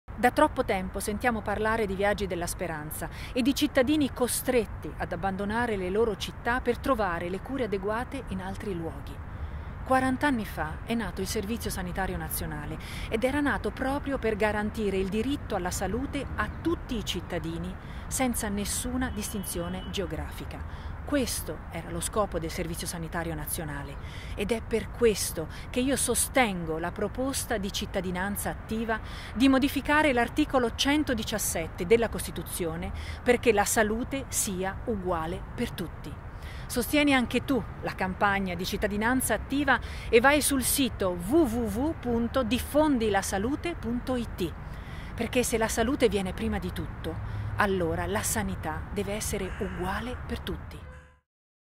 Alma Maria Grandin
"La salute è uguale per tutti", la presentazione della campagna di Cittadinanzattiva per affermare e rilanciare il diritto dei cittadini alla salute, che ad oggi appare tutelato in misura differente nelle varie aree del nostro Paese. La presentazione si terrà sabato 24 febbraio a Roma, dalle ore 10:30 presso la sala Igea dell'Istituto dell'Enciclopedia italiana in Piazza dell'Enciclopedia Italiana 4.
All'evento parteciperanno associazioni mediche e di pazienti, associazioni civiche, testimonial, costituzionalisti e interlocutori istituzionali a diverso titolo impegnati nella promozione di questo importante diritto.